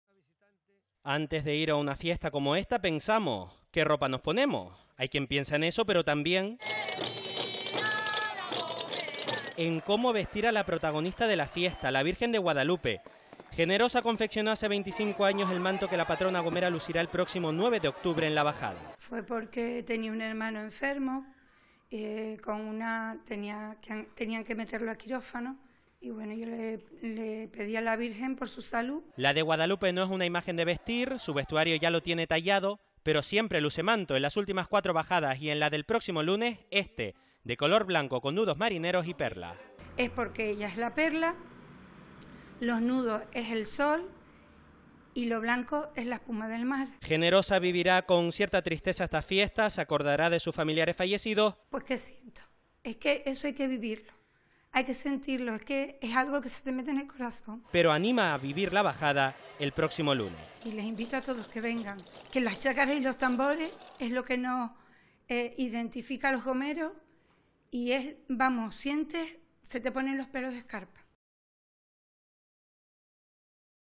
Manto de la Virgen de Guadalupe para las Fiestas Lustrales
Antes de ir a una fiesta como esta, pensamos en qué ropa ponernos y hay quien piensa en eso, pero también en cómo vestir a la protagonista de la fiesta, la Virgen de Guadalupe.  Generosa Herrera confeccionó hace 25 años el manto que la patrona gomera lucirá el próximo 9 de octubre en la Bajada. La de Guadalupe es una imagen que no necesita vestuario, está tallado en la imagen. Pero siempre luce manto. En las últimas cuatro bajadas y en la del próximo lunes es de color blanco con nudos marineros y perlas.

"Manto de / la Virgen"